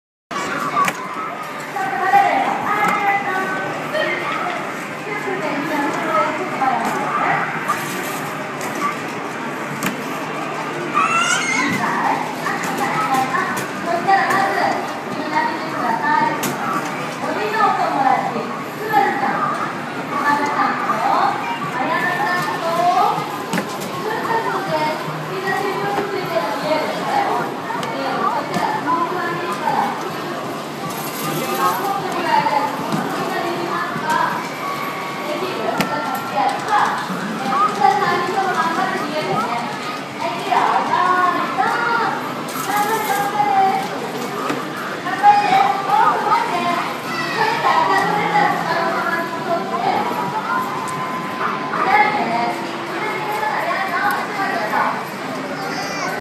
バイキンマンをやっつけた！
ずっとニヤけてる息子。楽しかったね。